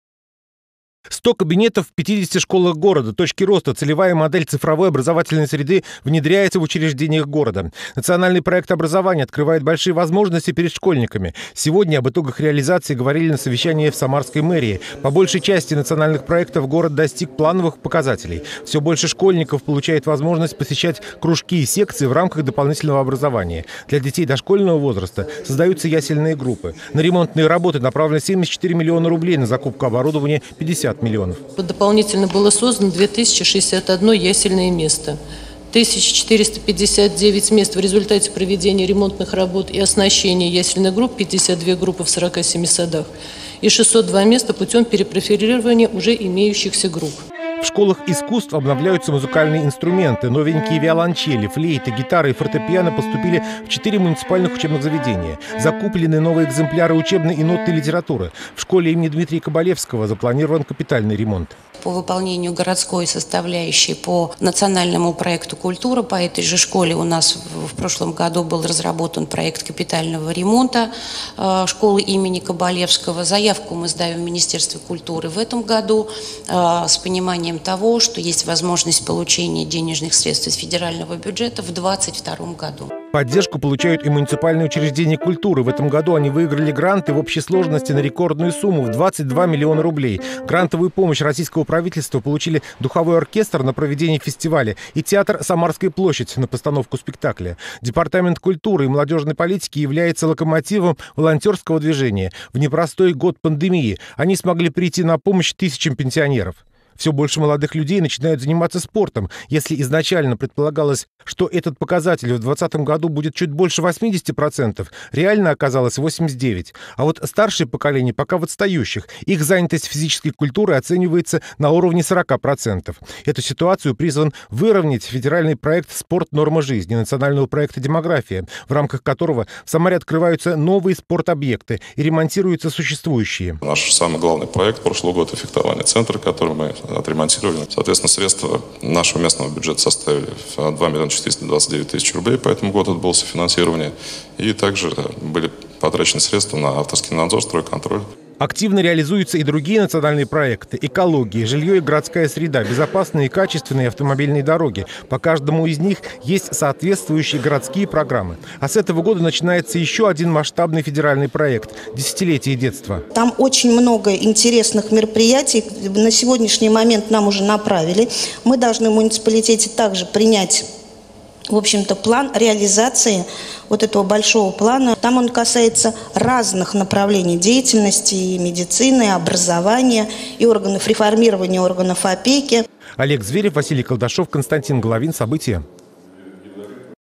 Национальные проекты открывают новые перспективы для развития регионов
Большие возможности для масштабных преобразований. Национальные проекты открывают новые перспективы для развития регионов. Сегодня в городской администрации подвели итоги их реализации за 2020 год. Благодаря федеральной и областной поддержке удалось многое сделать. Олег Зверев ознакомился с цифрами и фактами.